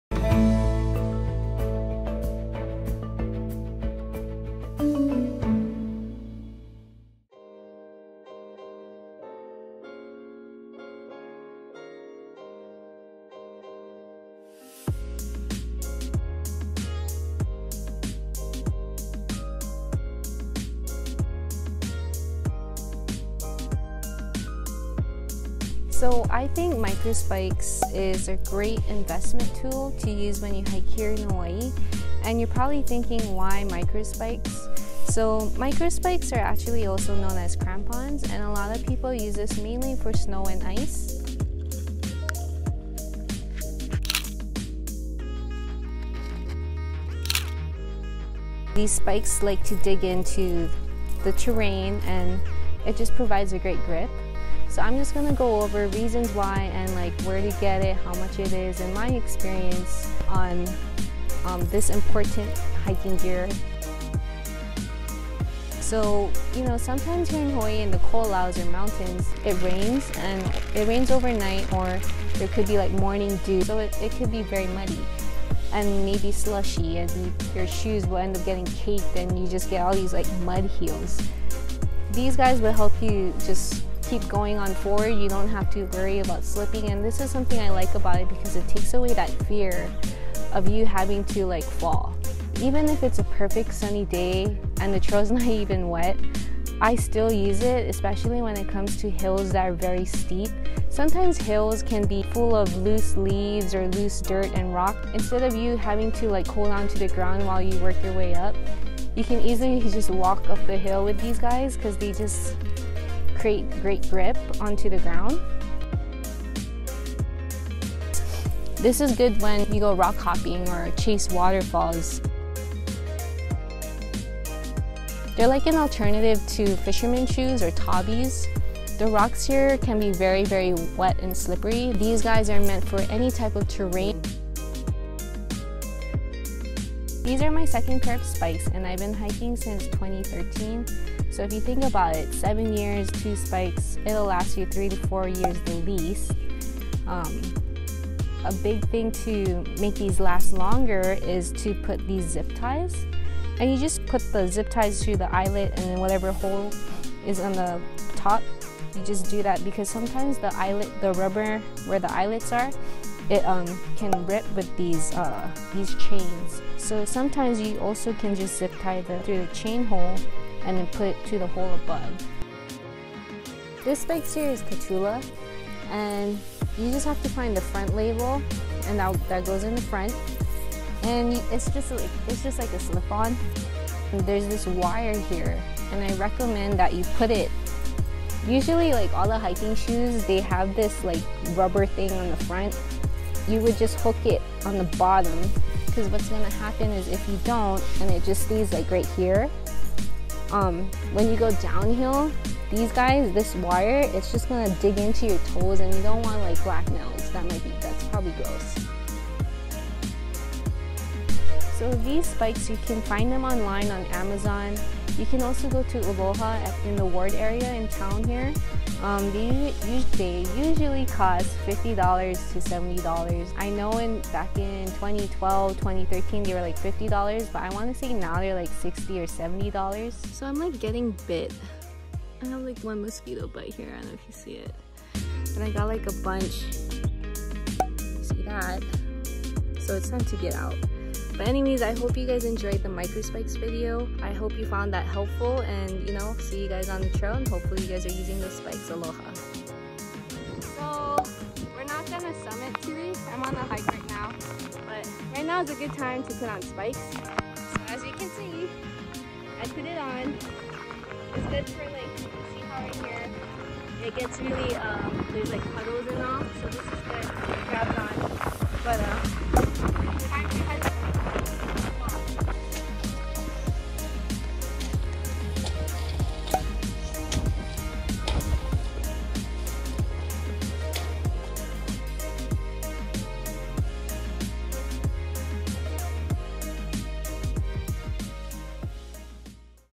Microspikes in Hawaii? An essential hiking gear to have
In this video, I will be going over one necessary item I think is great to have here on the Hawaii trails: Microspikes.